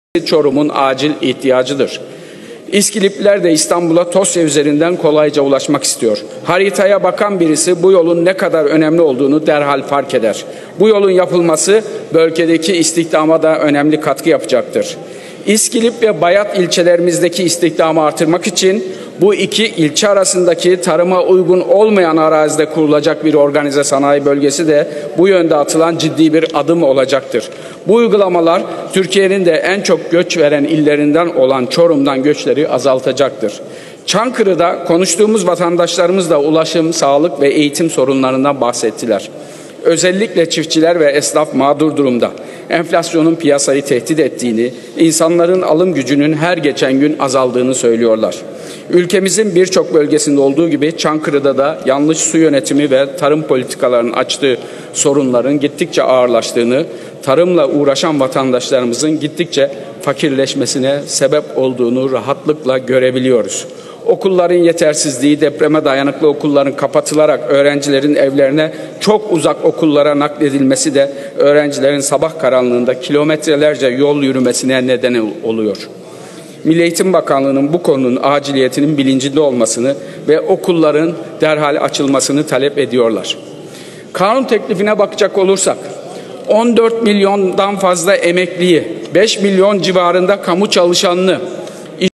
İSKİLİP ORGANİZE SANAYİ PROJESİ TBMM KÜRSÜSÜNDE
Geçtiğimiz hafta İyi Parti Ankara İl Başkan Yardımcısı Uğur Türkmen ile birlikte İskilip’i ziyaret eden İyi Parti Ankara Milletvekili Ayhan Altıntaş İskilip’in sorunlarını ve Organize Sanayi taleplerini Türkiye Büyük Millet Meclisi kürsüsünden vekillere anlattı.